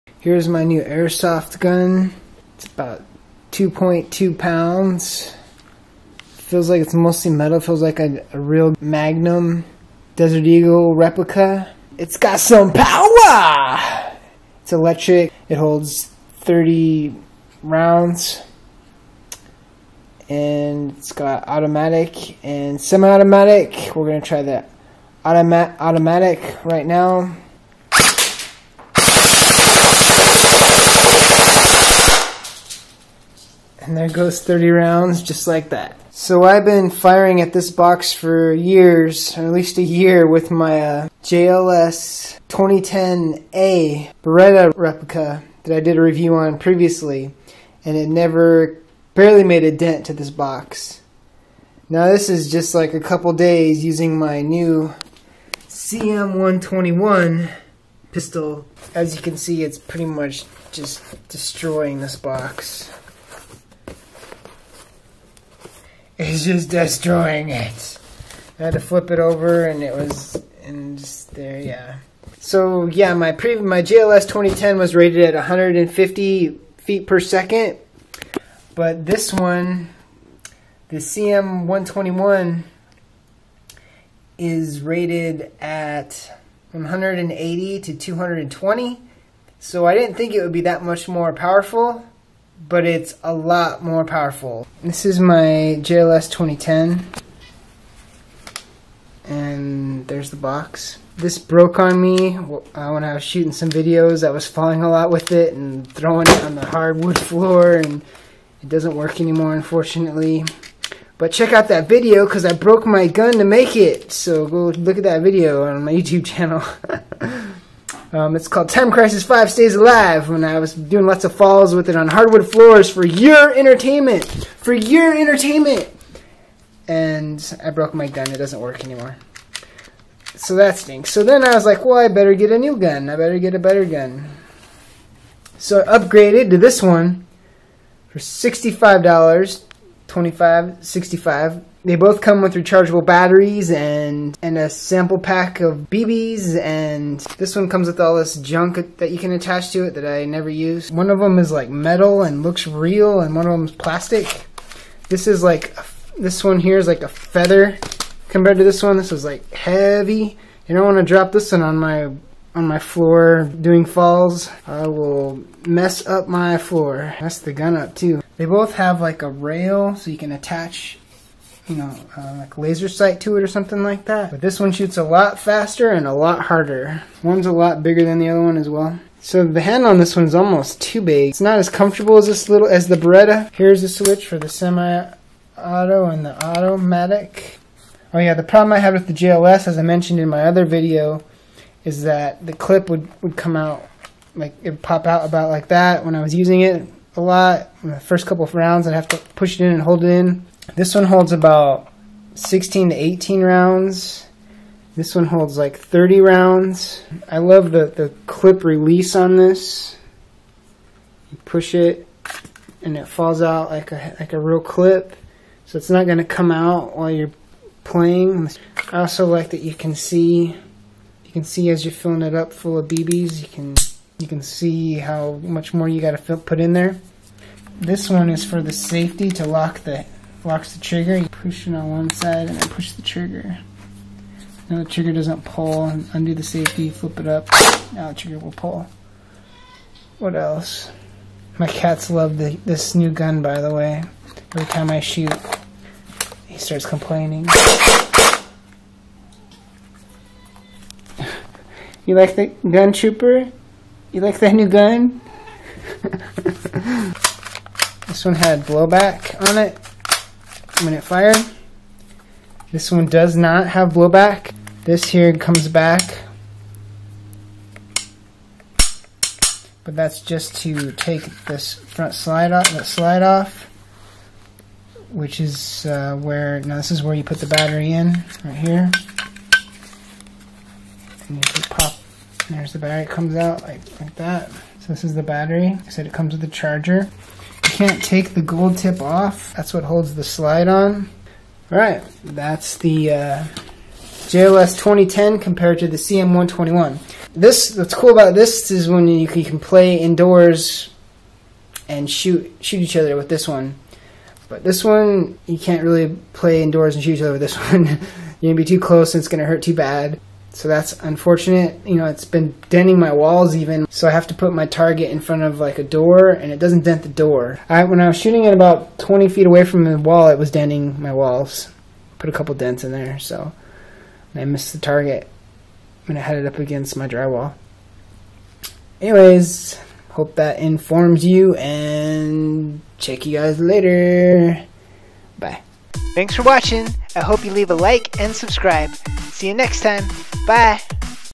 CM.121 $65 Electric Airsoft Pistol VS $25 Review Comparison
I compare my new CM.121 to my old JLS2010a. I also shoot and review the CM.121.